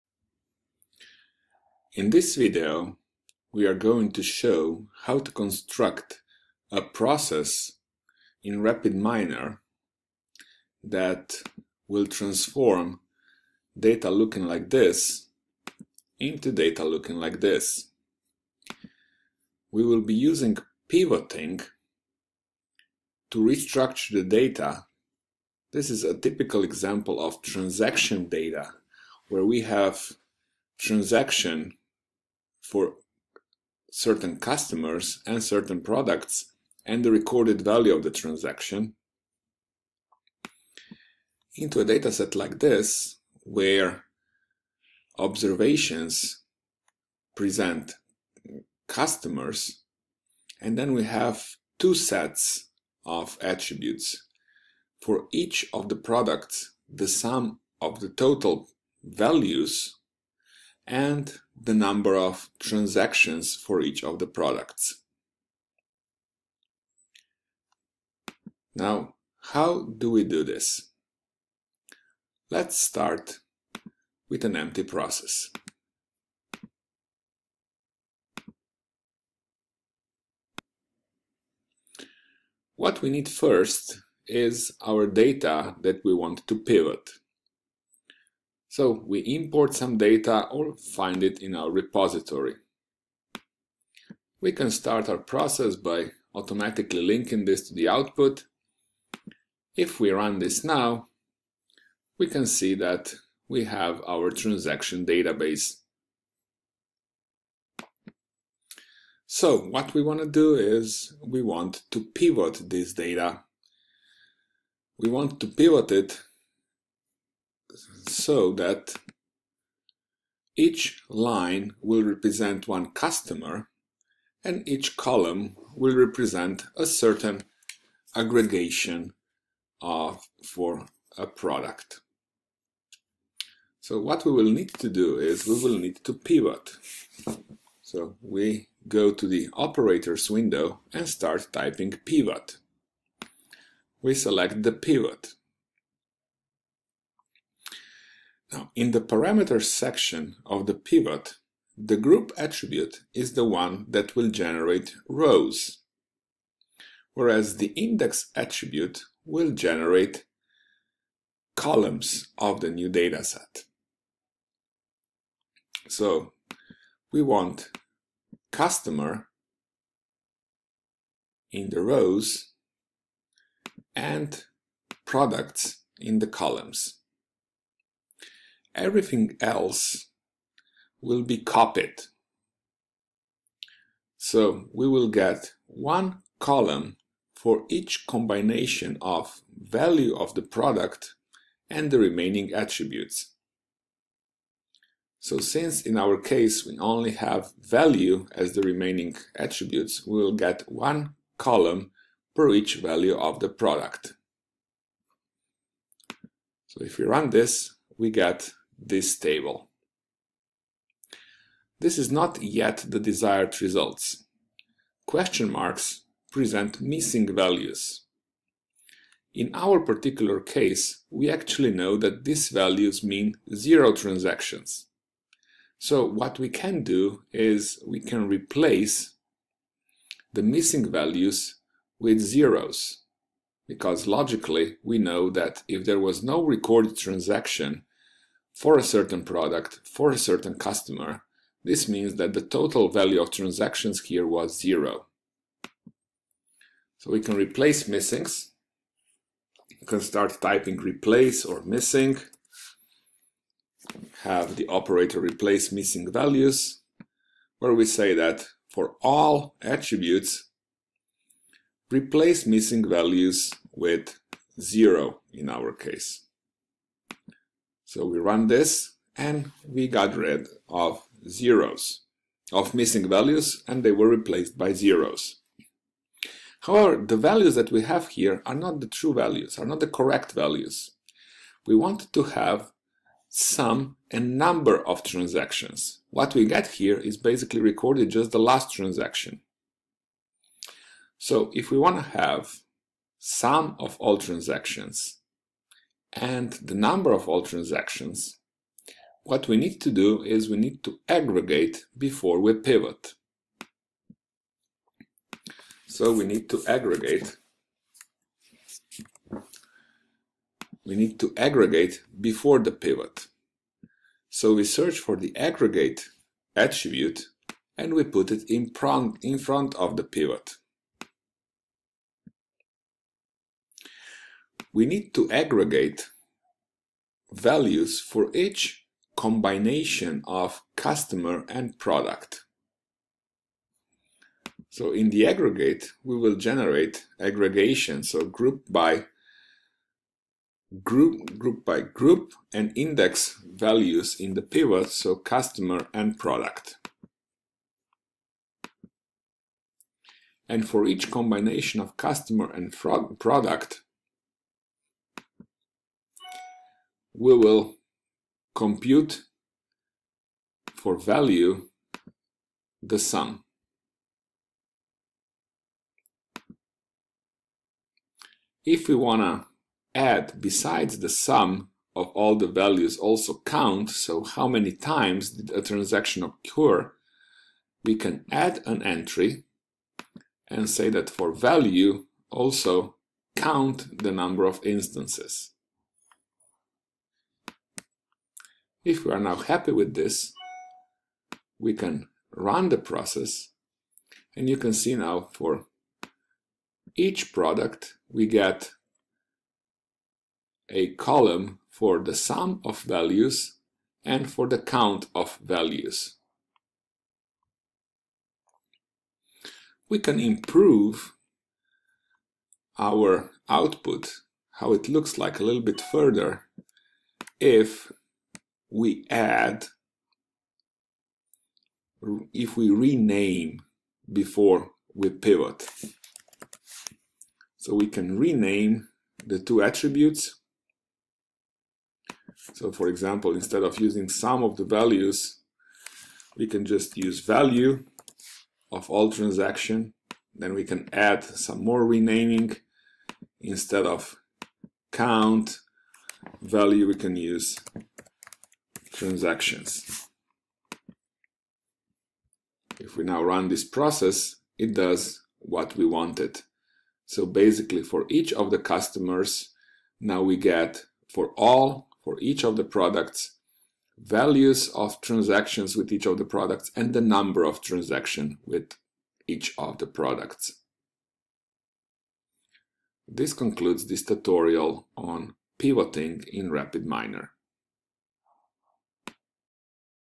Pivoting in RapidMiner
A short tutorial on how to pivot data in RapidMiner - an example of transaction data